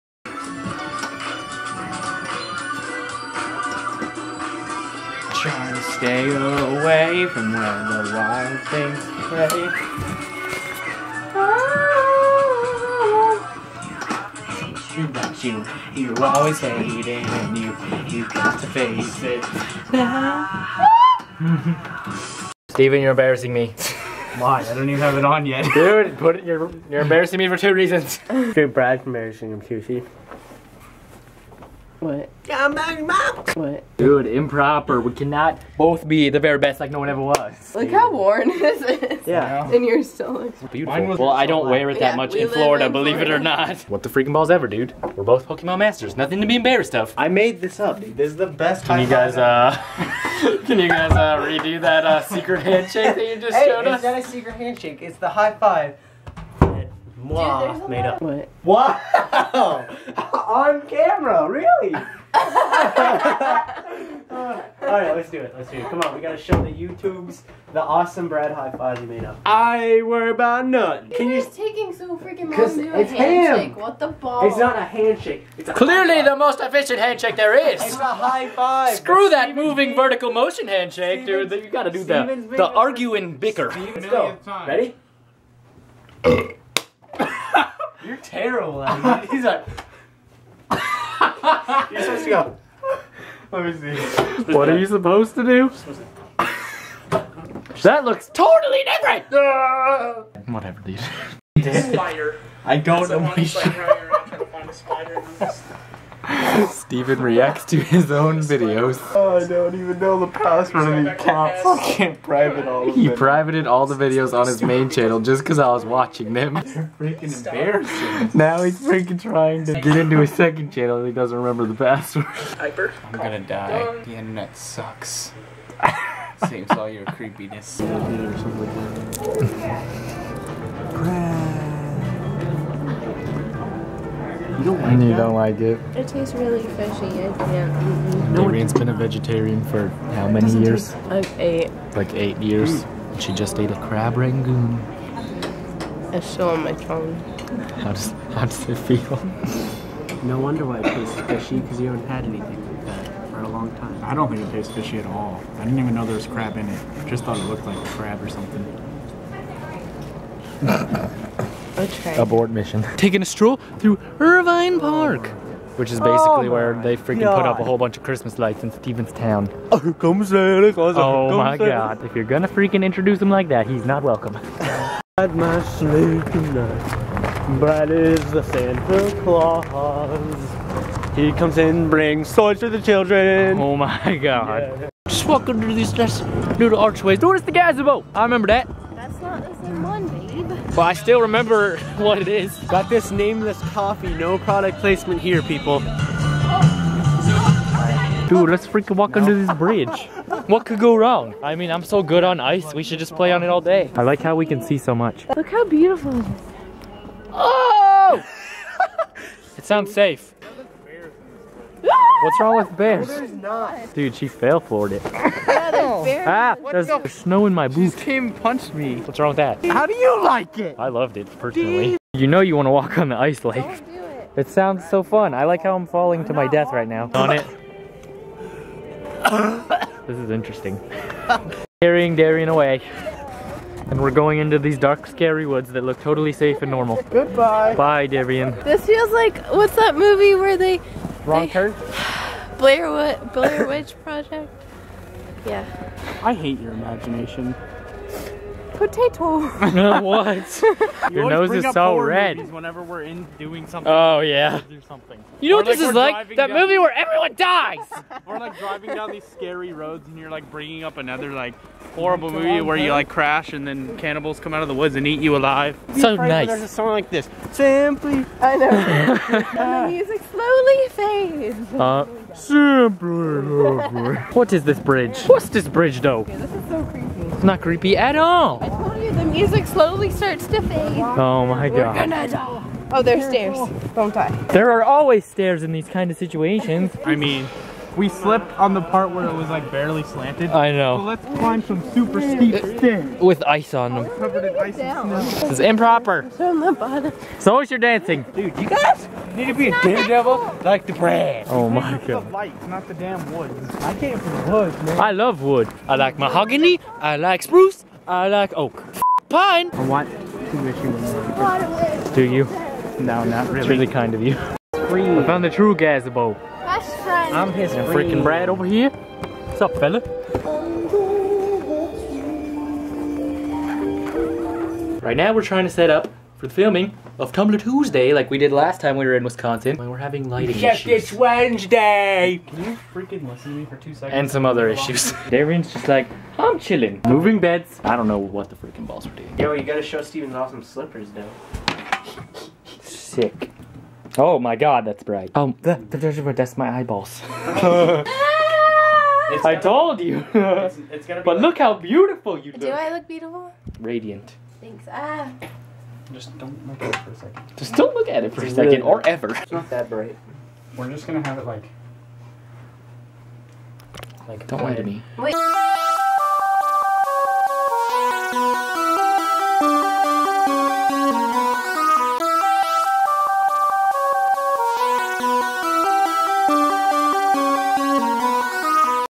IRVINE PARK CHRISTMAS LIGHTS
FAQ: Yes, all of these Wisconsin videos were filmed back in December. Sorry if you were confused :P haha.

Day 6 of Lonnie & Darian's trip to Wisconsin!

Today, Steven and Brad try to show us their unnecessarily complicated handshake and embarrass themselves. Then me and Steven wear the same AWESOME sweater and embarrass each other. And Darian tries to eat some sort of meat for the first time in YEARS... and nobody is really embarrassed... except maybe the crab.

At night time, we decided to check out the Irvine Park christmas lights! Apparently they're kinda well-known for their lights. It was pretty cool. The decorated the whole park and everything was lit up. If you're ever in Wisconsin during Christmas time, I'd recommend checking out Irvine Park.

Do it for the Vine. The ir. vine?